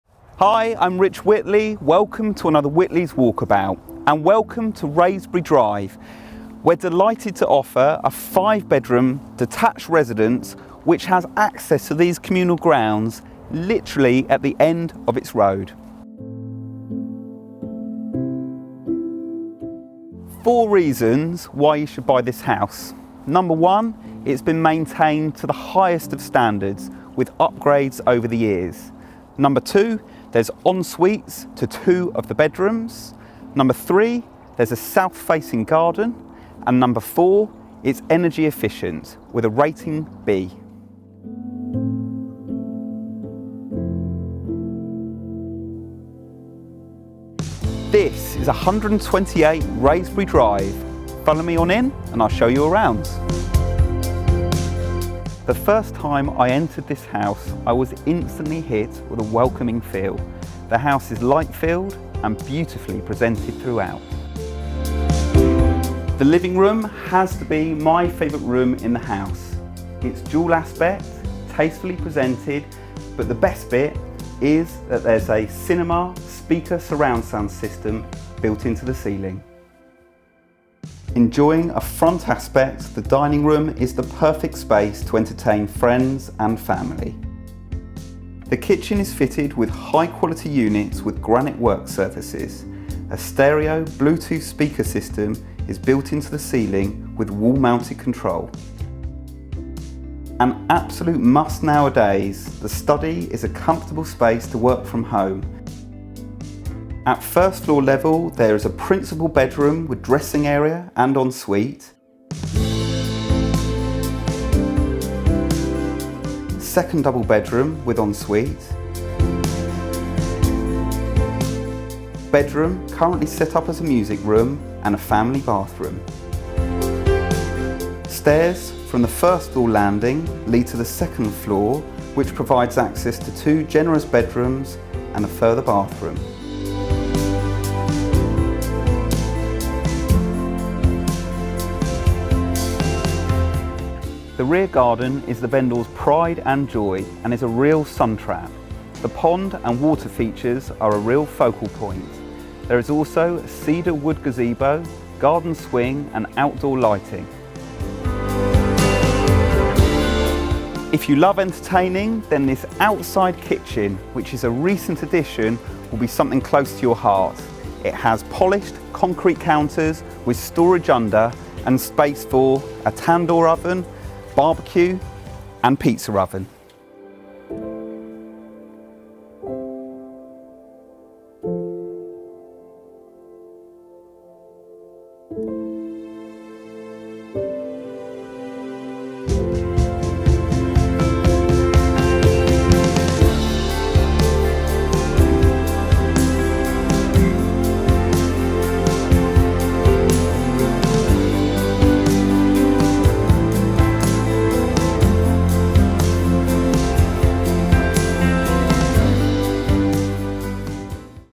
Wraysbury Drive, Yiewsley, West Drayton - Whitley's Walkabout Property Video Tour
Offering modern, luxury, ready-made comfort within the highly desirable Wraysbury Drive, this detached 5 Bedroom Residence with approximately 1906 sq ft of easy flowing accommodation will suit the needs of any larger or growing family.

✅ Dual Aspect Living Room With Surround Sound Speaker System
✅ Dual Aspect Dining Room
✅ Fitted Kitchen/Breakfast Room with Granite Worksurfaces & Stereo Bluetooth Speaker System
✅ Utility Room
✅ Study
✅ Principal Bedroom With Dressing Room & En-Suite
✅ Driveway & Integral Garage
✅ Landscaped South Facing Rear Garden
✅ Multiple TV & Cabled Network Points
✅ External CCTV & Alarm System
✅ Solar Panels To Roof

To arrange a viewing call our sales team on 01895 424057!